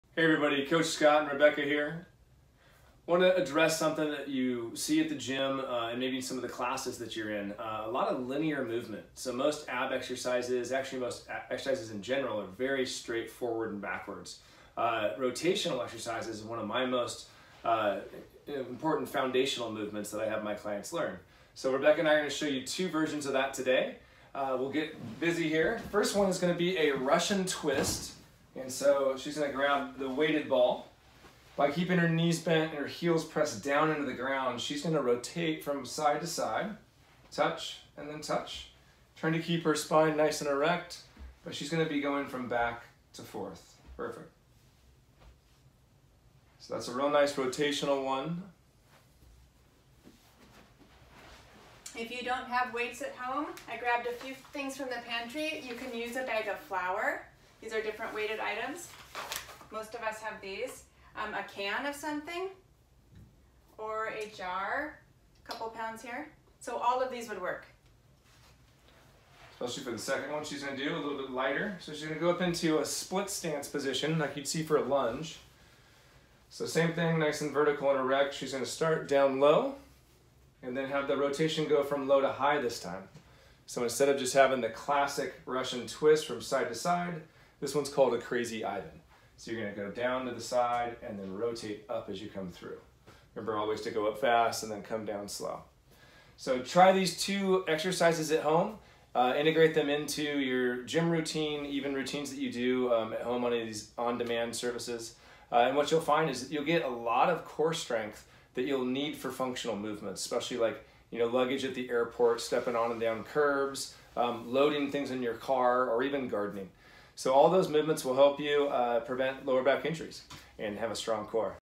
Rotational Core Exercise
Stay strong physically and help your mental strength during this time!
One of the first foundational movements I teach my clients is rotational core. If you notice almost all movements done in gyms or classes are linear. Life doesn’t move in straight lines!!⠀
Most lower back injuries happen when you lift and rotate at the same time, especially with load. Examples of this are: gardening, loading items in your car, or picking items up off the ground.⠀
Prepare yourself for life’s challenges and improve your functional core strength. ⠀
We have enough adversity with the current coronavirus. Let’s not add on injury too!⠀
Try these two rotational core movements you can do anywhere.